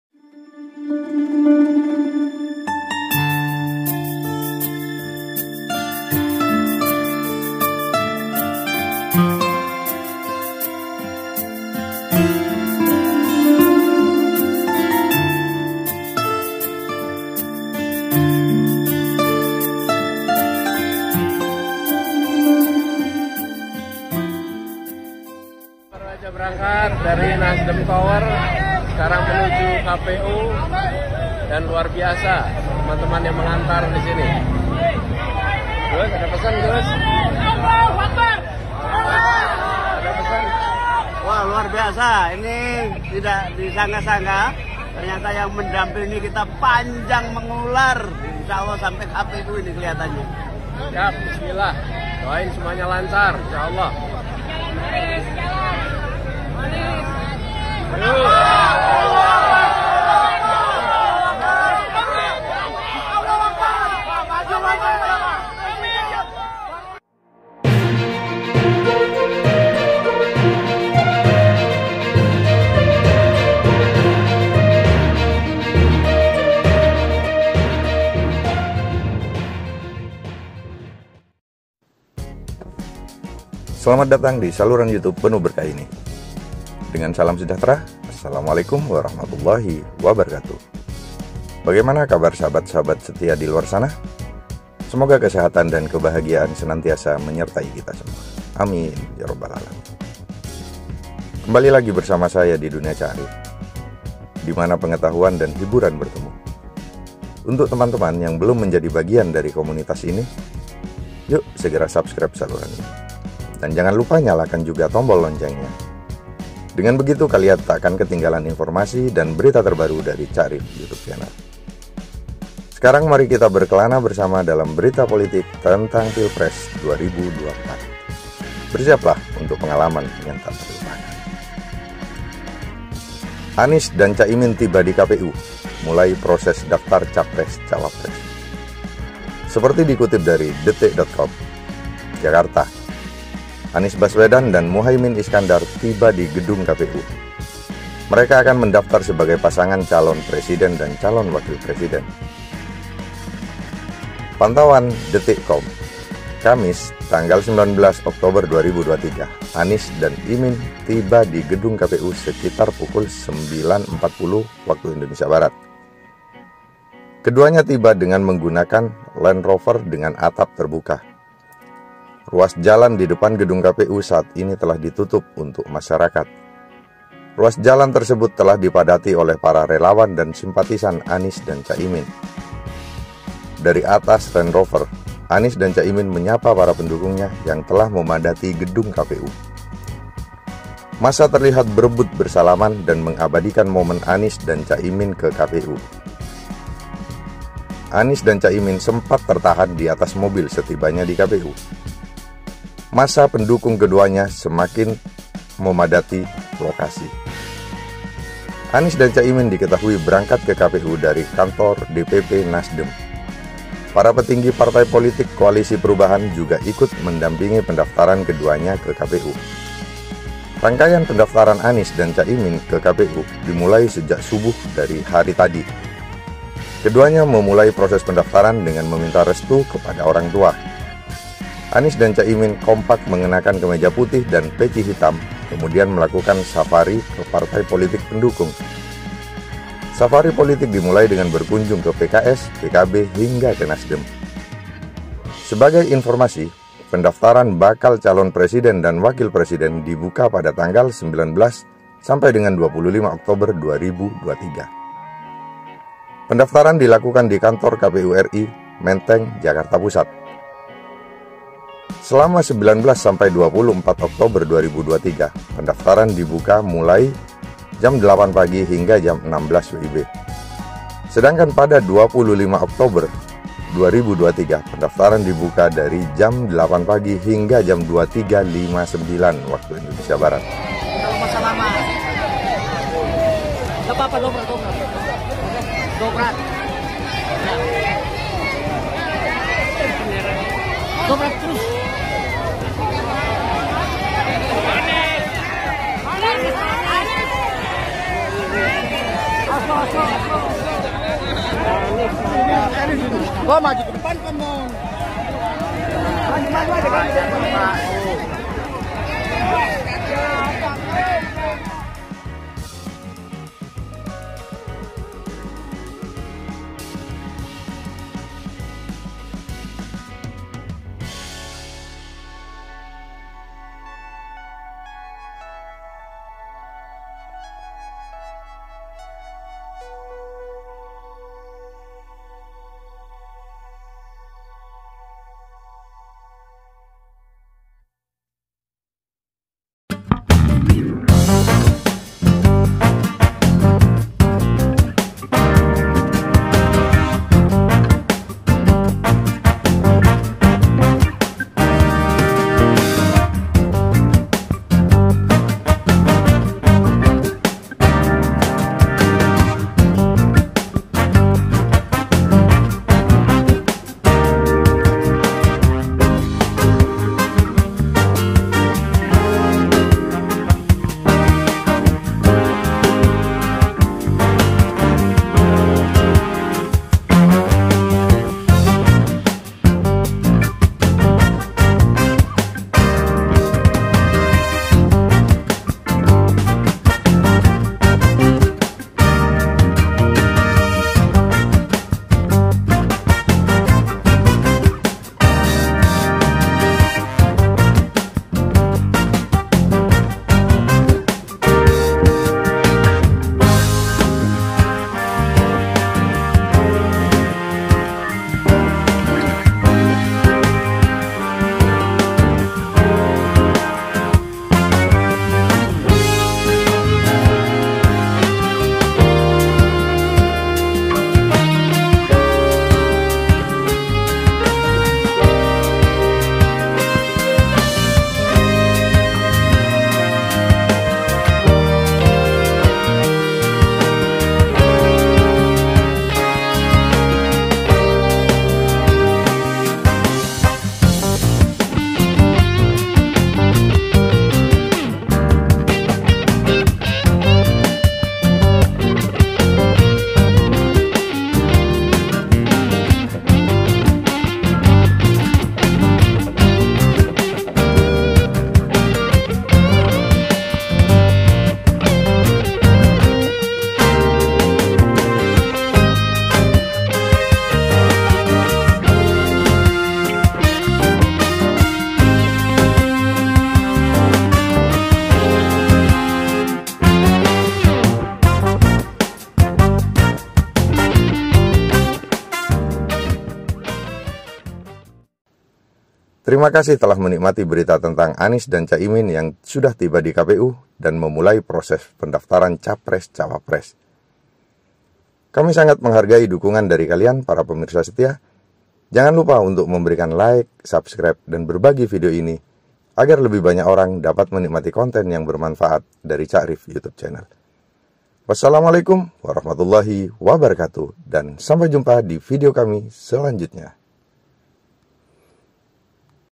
LUAR BIASA!!! ANIES BASWEDAN - CAK IMIN diiringi jutaan orang daftar ke KPU.
Anies dan Cak Imin Tiba di KPU, Mulai Proses Daftar Capres-cawapres.||

Jakarta - Anies Baswedan dan Muhaimin Iskandar tiba di gedung KPU. Mereka akan mendaftar sebagai pasangan calon presiden dan calon wakil presiden.||
Pantauan detikcom, Kamis (19/10/2023), Anies dan Imin tiba di gedung KPU sekitar pukul 09.40 WIB. Keduanya tiba menggunakan Land Rover dengan atap terbuka.||

Ruas jalan di depan gedung KPU saat ini telah ditutup untuk masyarakat. Ruas jalan tersebut telah dipadati oleh para relawan dan simpatisan Anies dan Cak Imin.||

Dari atas Land Rover, Anies dan Cak Imin menyapa para pendukungnya yang telah memadati depan gedung KPU. Massa terlihat berebut bersalaman dan mengabadikan momen Anies dan Cak Imin ke KPU.||

Anies dan Cak Imin sempat tertahan di atas mobil setibanya di KPU. Massa pendukung keduanya terlihat semakin memadati lokasi.||

Anies dan Cak Imin diketahui berangkat ke KPU dari kantor DPP NasDem. Para petinggi partai politik Koalisi Perubahan juga ikut mendampingi pendaftaran keduanya ke KPU.||

Rangkaian pendaftaran Anies dan Cak Imin ke KPU dimulai sejak subuh hari tadi. Keduanya memulai proses pendaftaran dengan meminta restu kepada orang tua.||

Anies dan Cak Imin kompak mengenakan kemeja putih dan peci hitam kemudian melakukan safari ke partai politik pendukung. Safari politik dimulai dengan berkunjung ke PKS, PKB hingga ke NasDem.||